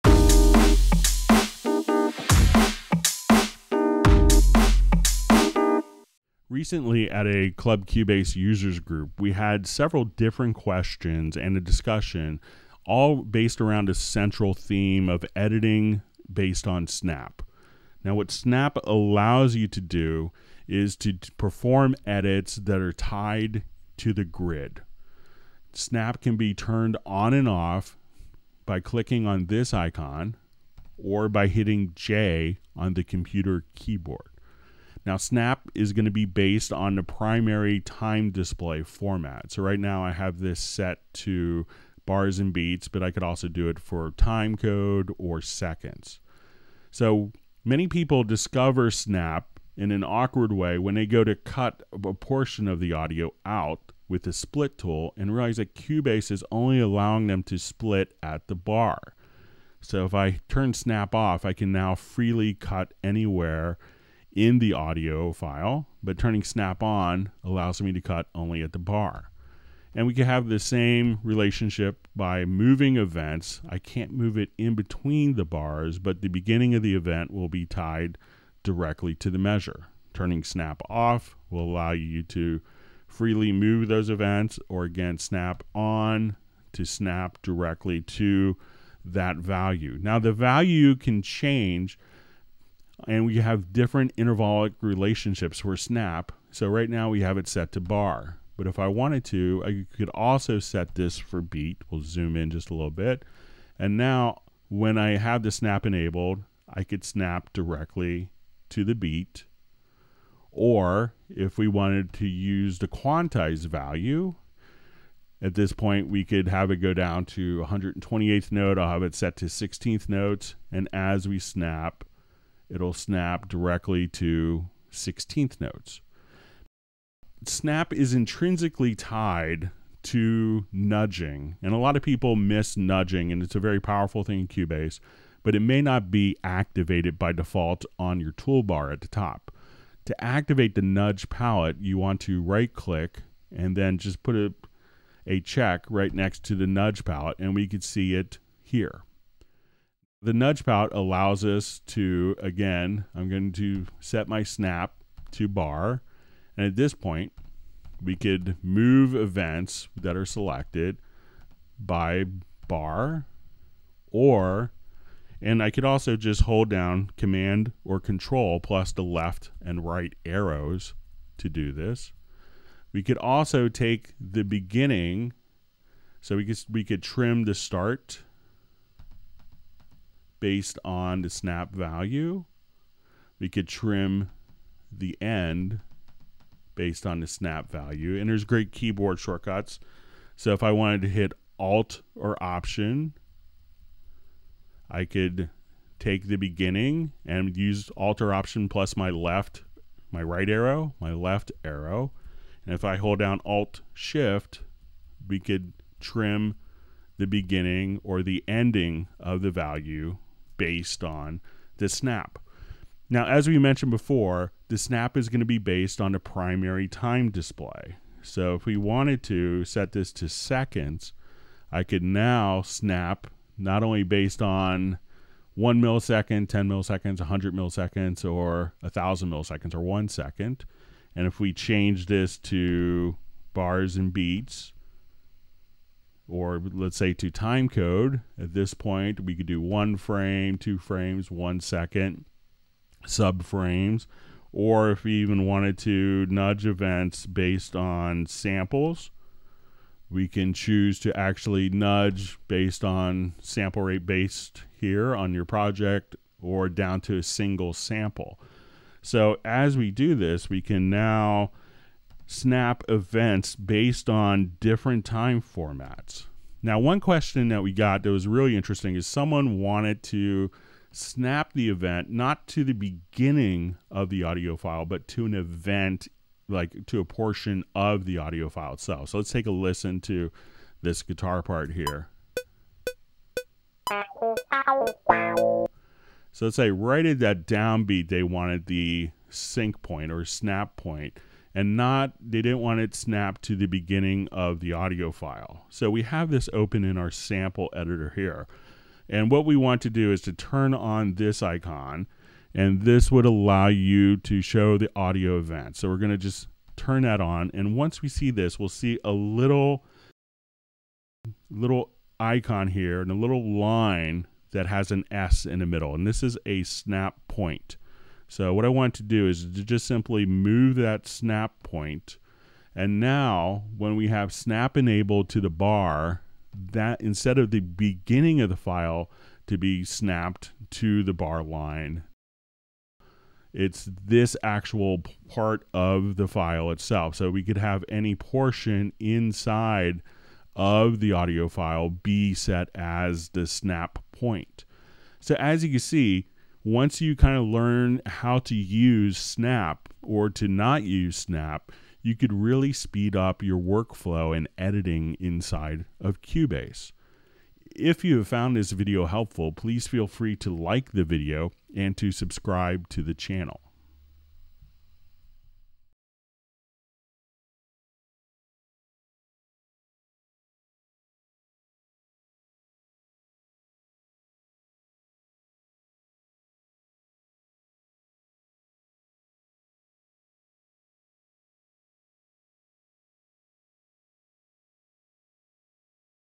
How to Use the Snap Function in Cubase | Q&A with Greg Ondo
What is the snap function available Cubase and how can it help you? Well Greg’s there to elaborate on editing based on snap. You get to find out how to work with snap, how to turn the snap function on and off, and there are many useful examples of cutting and editing audio with snap.